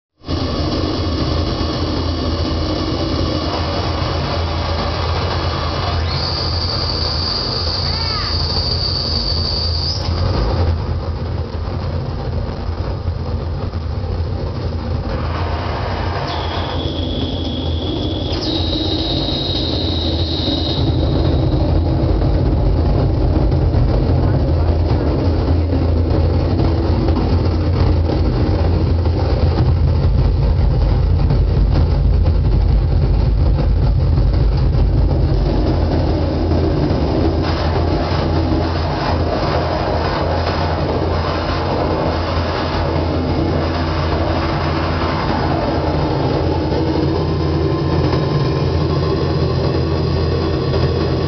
Don Haugen and [view] LIVE @ What is Noise Fest Day 2
Just a short clip before my battery died, but a KILLER set of harsh textures and noise from Don Haugen and [view] @ What is Noise Fest day 2, 2019 in Corvallis Oregon USA.